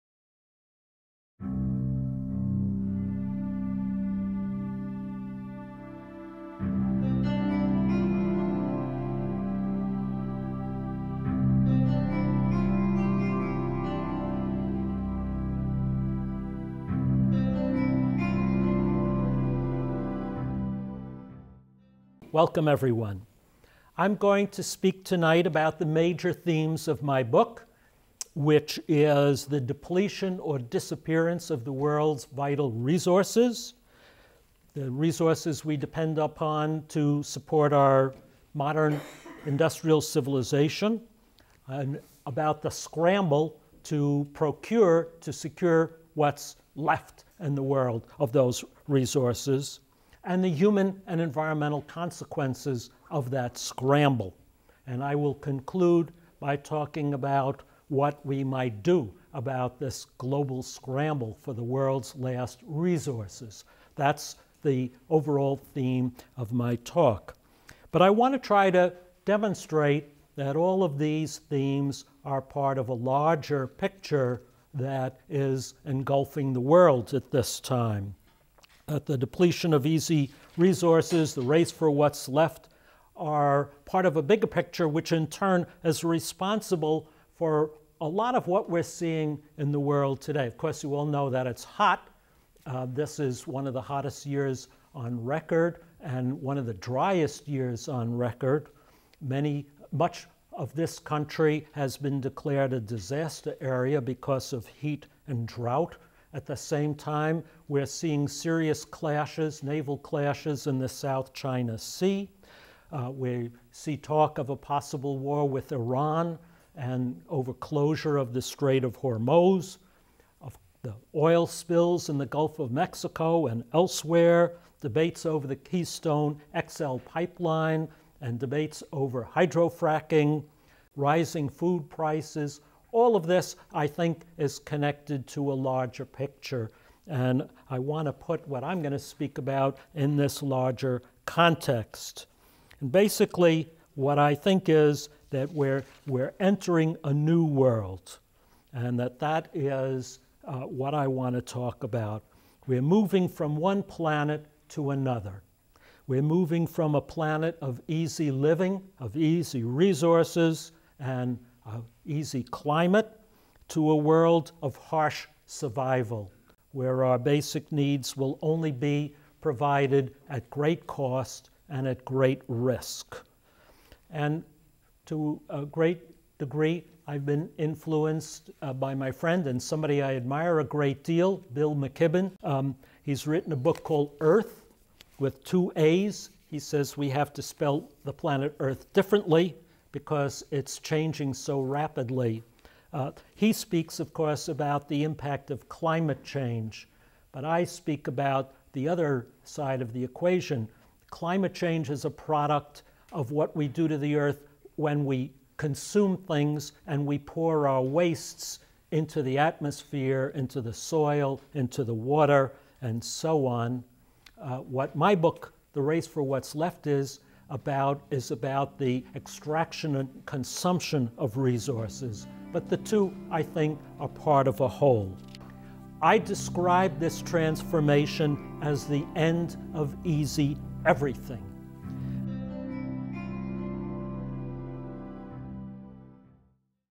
The Race for What’s Left: The Global Scramble for the World’s Last Resources (trailer)
Renowned energy expert Michael T. Klare provides an invaluable account of the new and increasingly dangerous competition for the world's dwindling natural resources. Arguing that the world is facing an unprecedented crisis of resource depletion -- one that goes beyond "peak oil" to encompass shortages of coal and uranium, copper and lithium, water, and arable land -- Klare shows how the desperate hunt for raw materials is forcing governments and corporations to stake their claim in ever more dangerous and remote areas that present grave political and environmental risks.

Citing mounting tensions between the U.S. and China over control of resources in the Asia-Pacific region, volatile local border disputes that raise the likelihood of military confrontation, and the destructive environmental consequences of tar sands oil extraction and fracking, Klare argues that we need to radically alter our consumption patterns and build alternative energy systems before it's too late.